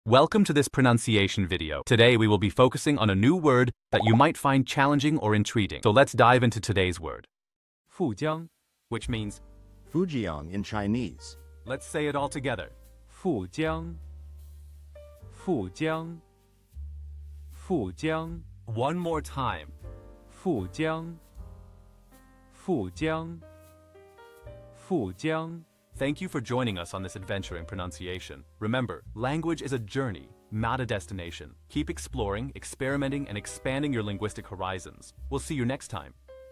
How to pronounce fù jiāng | 富江 (Fujiang in Chinese)
Master the Pronunciation of 'fù jiāng | 富江' - which means : Fujiang in Chinese 📝 with @PerfectPronunciationChinese 🎓🗣️ - [Your Guide to Chinese]

Learn to pronounce 'fù jiāng' perfectly in Chinese with our easy and engaging tutorial by @PerfectPronunciationChinese! 📖📽️ In this comprehensive video, we provide you with native speaker examples 🗣️👂 and phonetic breakdowns 🎵 to ensure you get the pronunciation just right.